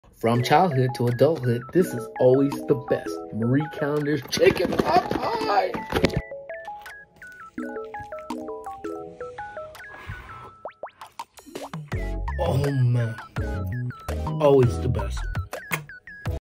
Chicken Pot Pie Happiness! 😋😃
I've been eating this Chicken Pot Pie from Marie Calendars since I was a little tyke. Well not the exact one in my hand, but you know what I'm trying to say. Honestly, it's probably one of the best microwavable meal in the game. If you're a fan of a good and cheap Chicken Pot Pie, this one is for you! Let me know what you think! Give us a sub and follow our foodie adventures!